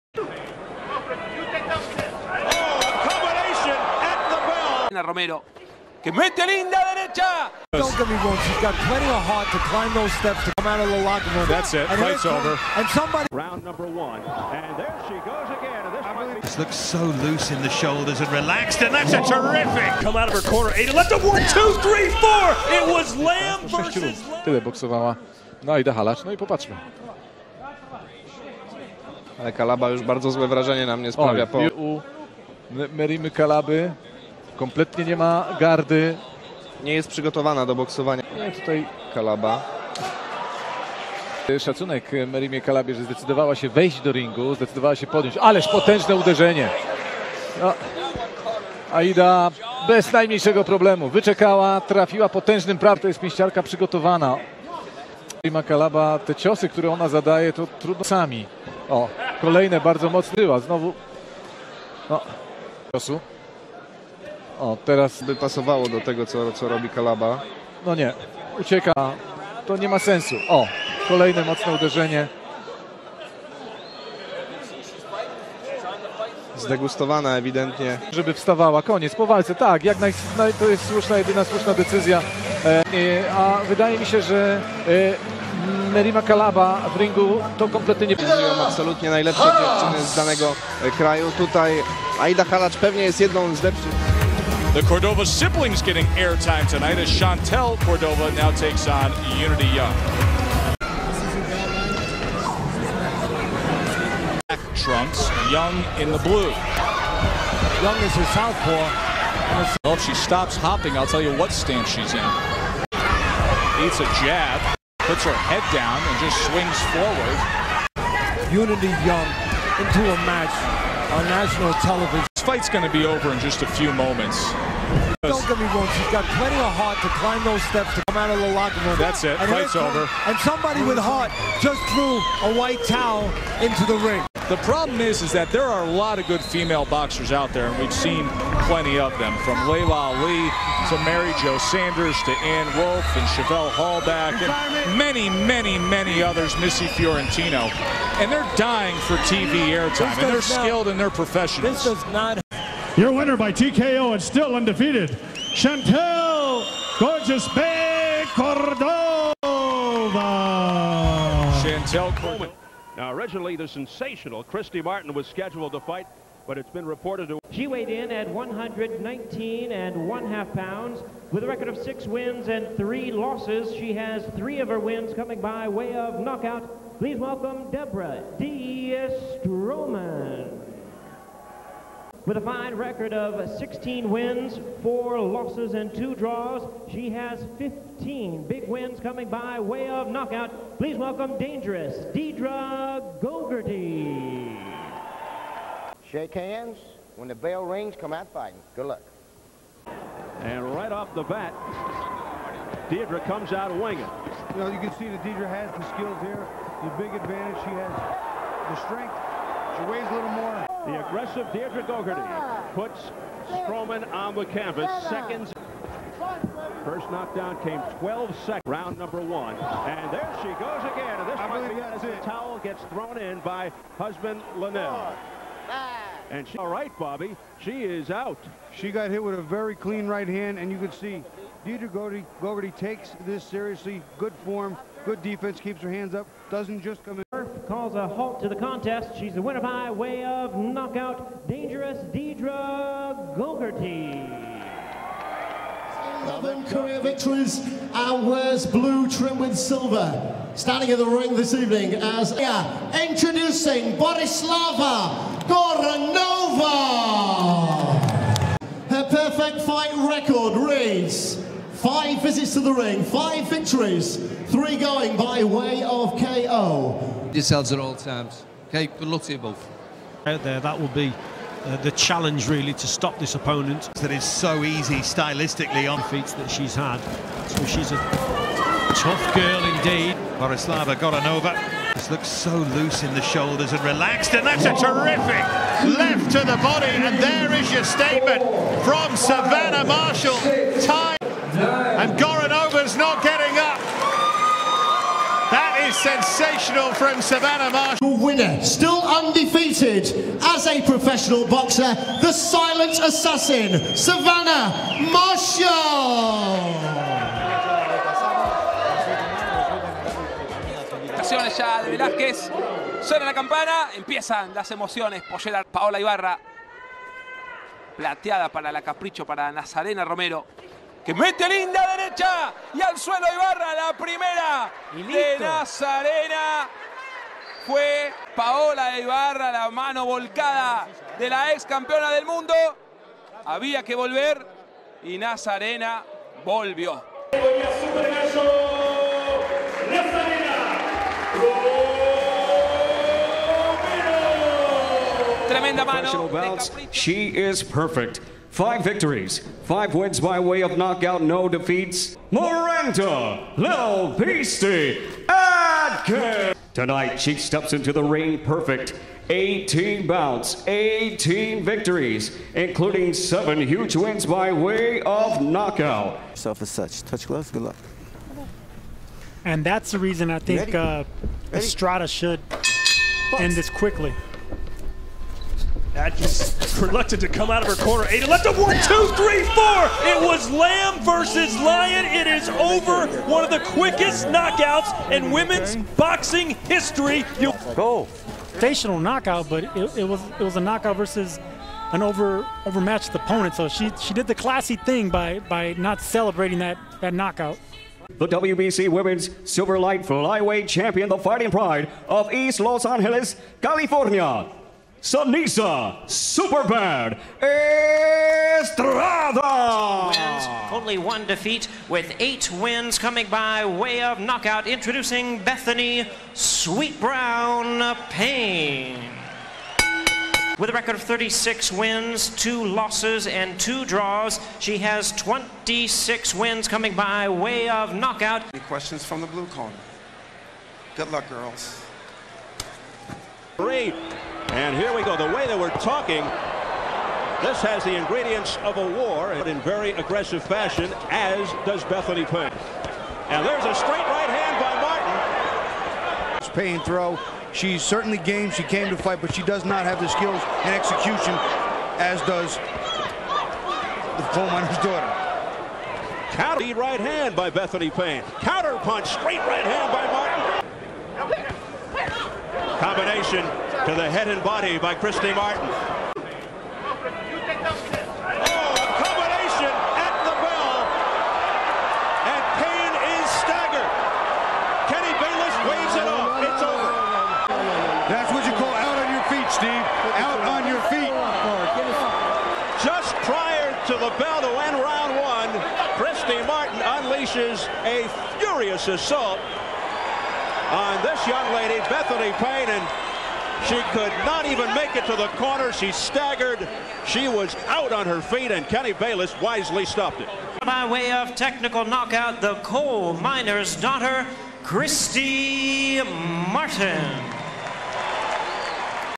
Female Boxing Fights that should have never allowed to happen
These female fights should have never allowed to happen because they were  one-sided. They were the result of ridiculous match making and the results were indeed a disaster.

Fights in this video:
-Aida Halac vs Merima Kalaba
- Chantel Cordova vs Unity Young
-Diedre Gogarty vs Debra Stroman
-Savannah Marshall vs Borislava Goranova
-Nazarena Romero vs Paola Ibarra
-Seniesa Estrada vs Miranda Adkins
-Cristy Martin vs Bethany Payne